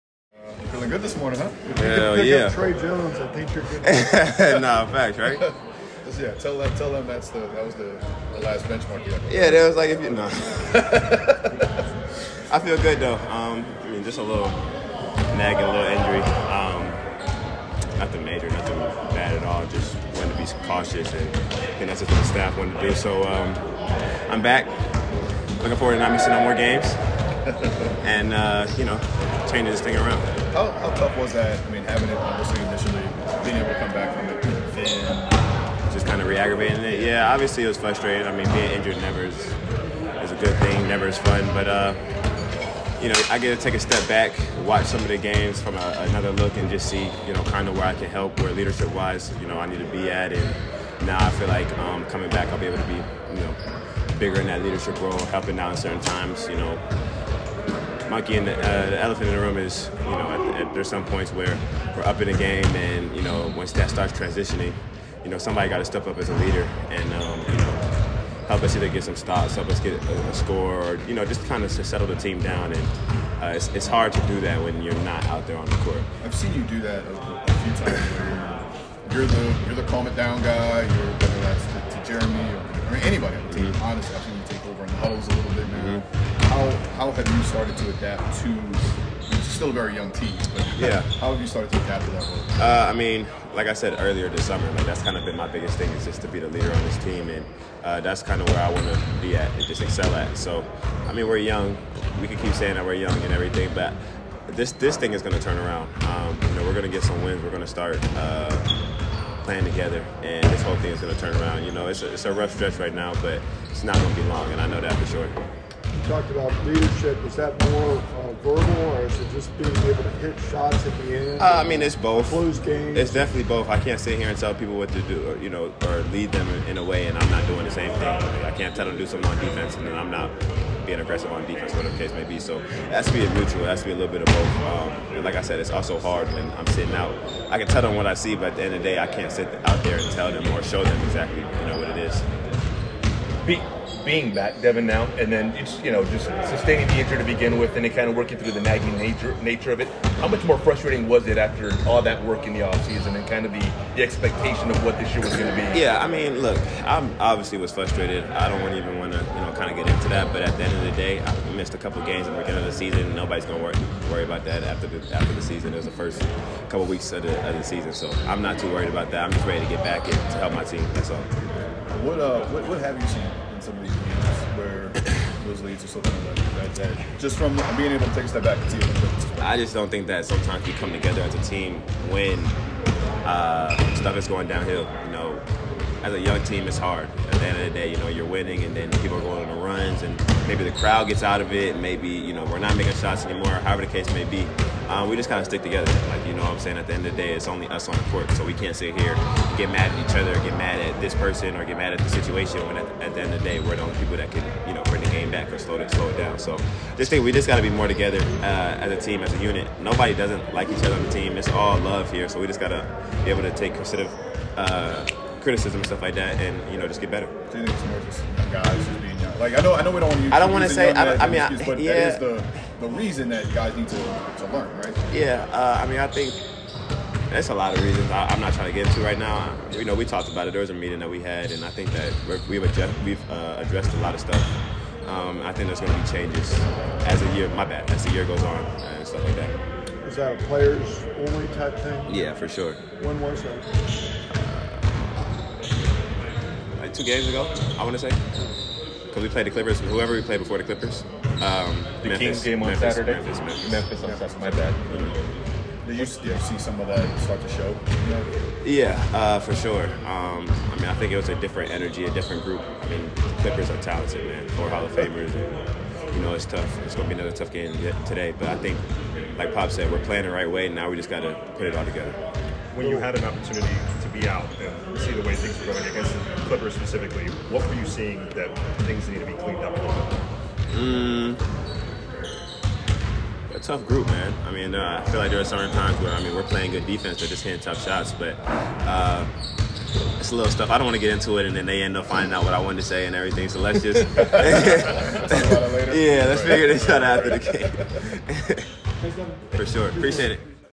2023-24 San Antonio Spurs Season | Devin Vassell Shootaround vs LA Clippers 11.22.2023
San Antonio Spurs Devin Vassell media availability after Spurs Practice on November 22, 2023 at Victory Capital Performance Center.